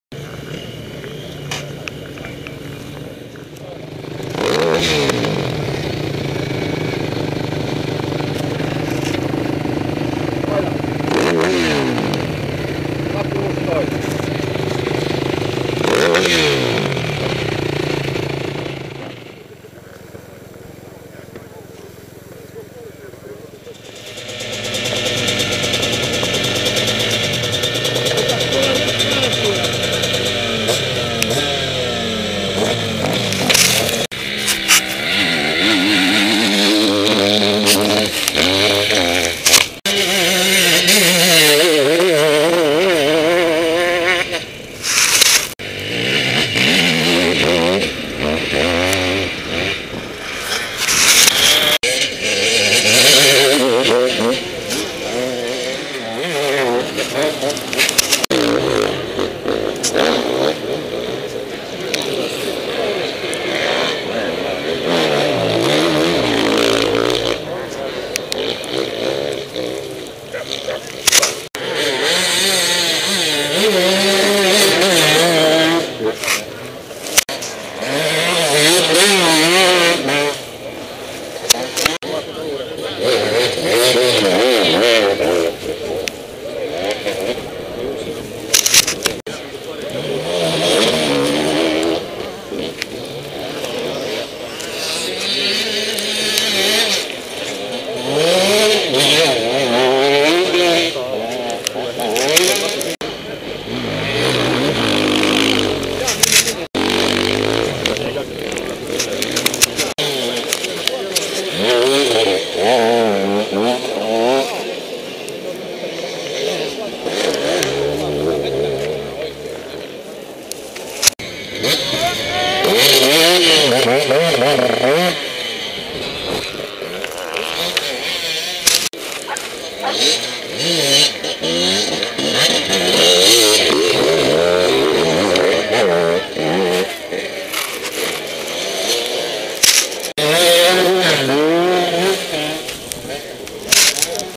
Karkkilan endurolenkki A/B-luokat 26.2.2012
A-luokan lähtöä. Roni Nikander, Markus Michelsson, Olli Kuhanen, Niko Kalatie.
Lopussa B-luokkaa reitin alkupuolella.